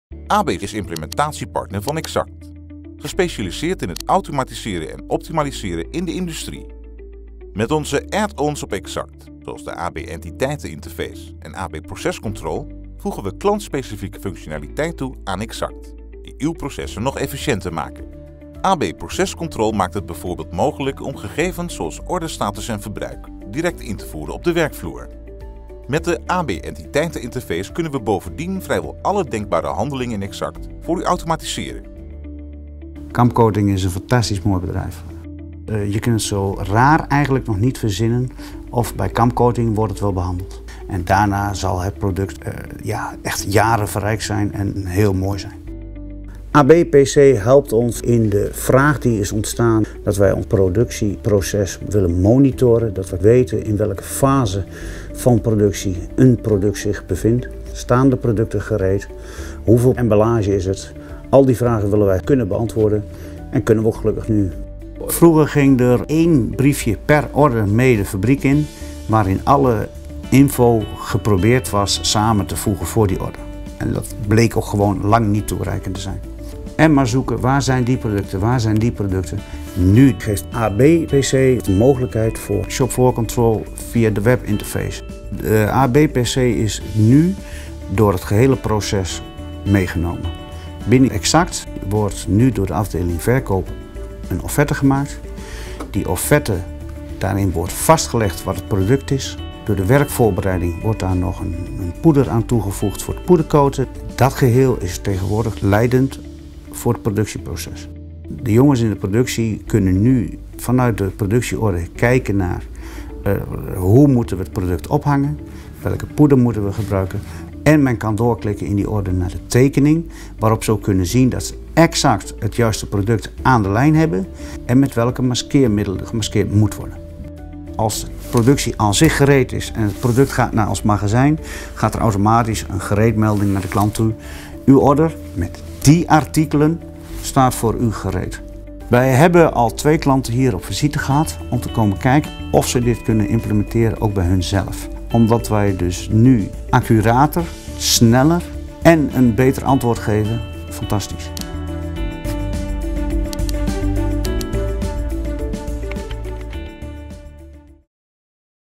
Kamp Coating gebruikt AB Manufacturing Control
Kamp Coating registreert de voortgang op de werkvloer met AB Manufacturing Control.